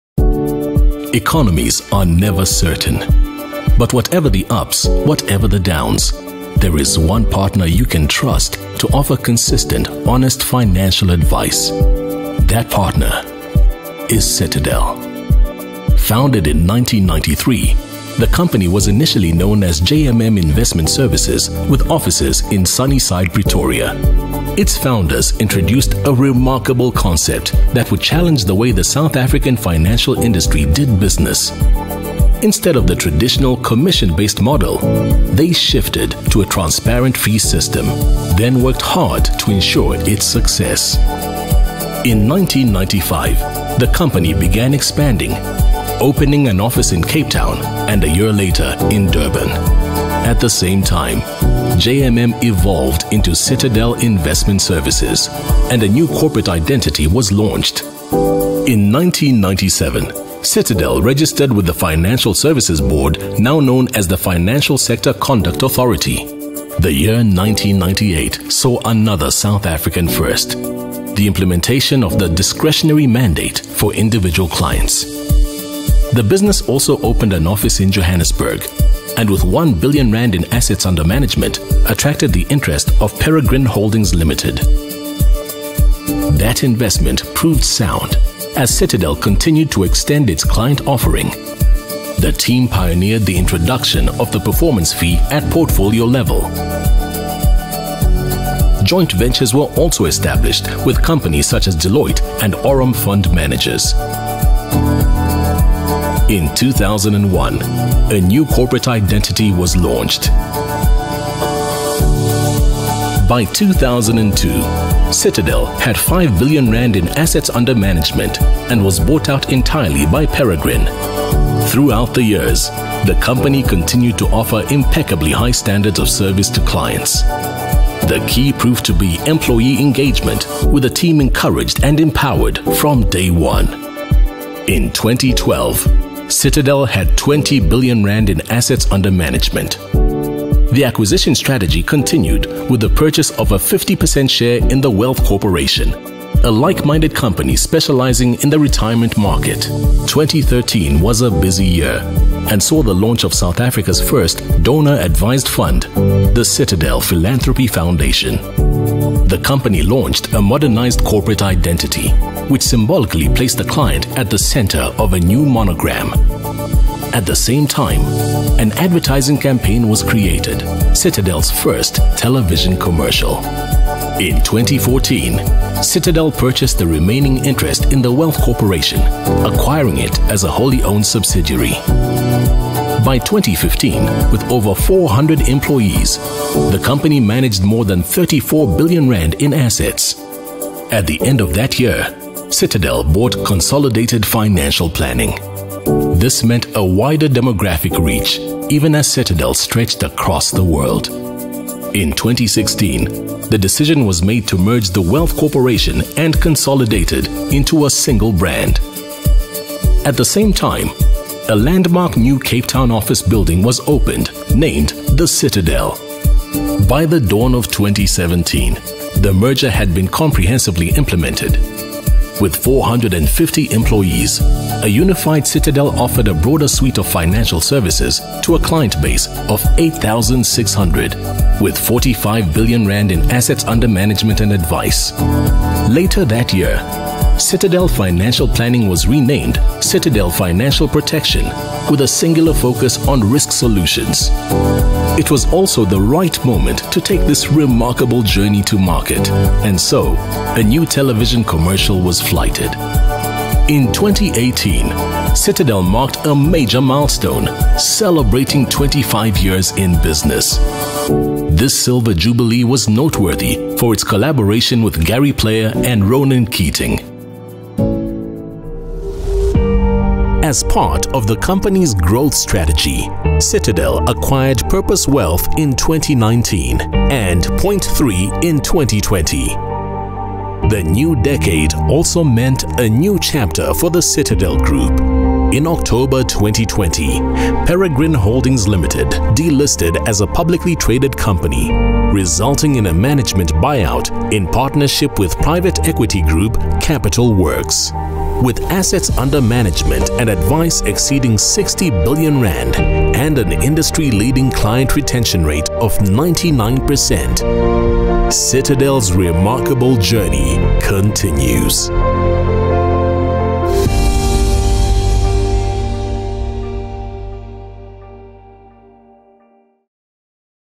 Our story
Citadel is a specialist wealth management company based in South Africa, with offices across the country and one in Guernsey. Advice is the cornerstone of the Citadel offering, not only where to invest, but, more importantly, why. The company was founded in 1993, has 451 employees and more than R70 billion in assets under management and advice. Citadel’s key differentiator in an increasingly crowded market is our unwavering client-centric approach. As a result of this approach, more than 99% of clients who invest with us, stay with us.